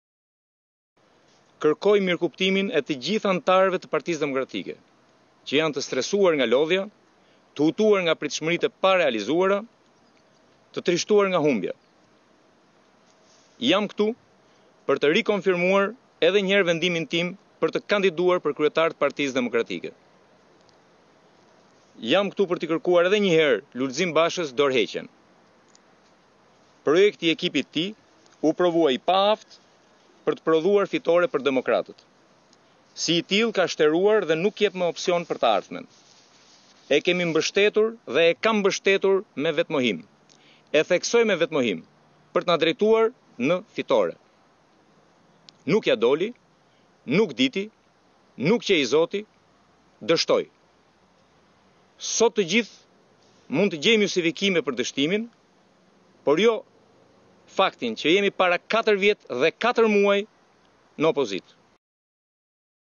Top Channel/ “Basha dështoi!”, Fatbardh Kadilli: I paaftë për fitoren, të japë dorëheqjen
Fatbardh Kadilli, anëtar i Kryesisë së Partisë Demokratike kërkon të kandidojë për vendin e kreut të partisë, duke i kërkuar kreut aktual, Lulzim Basha të japë dorëheqjen.

Kjo kërkesë e tij vjen pas dështimit për herë të tretë të PD-së në zgjedhje, ku fitues ka dalë PS.

Kadilli ka kërkuar mirëkuptimin e të gjithëve për këtë kërkesë, duke theksuar se Basha ka dështuar me planet e projektin e tij.

“Është vendosur një barrikadë para portës ku Lul Basha u betua se s’do dorëzohet, por është e tepërt, kërkoj kuptimin e PD-së, anëtaret e saj janë të trishtuar nga humbja, jam këtu për të konfirmuar edhe njëherë, jam këtu për t’i kërkuar edhe njëherë Bashës dorëheqjen, projekti i tij është i paaftë, e kemi mbështetur dhe e kam mbështetur me vetëmohim për të na drejtuar në fitore, nuk ia doli nuk diti nuk qe i zoti,  dështoi. Nuk e dimë se çfarë na pret në fund, sot kush thërret Rama ik nuk mund të thotë Basha rri. Dështimi i Bashës nuk mund t jetë për të mos luftuar Ramën. Këtë deklaratë të re do të doja ta bëjë në mjediset e PD, kam kërkuar nga zyra e shtypi, nuk dua të hyj në partinë tim vjedhurazi por si një njeri që ka hapësirën e vet në një shtëpi të lirisë. Çdo demokrat ka të drejtën tu flasë qytetarëve”, tha Kadilli.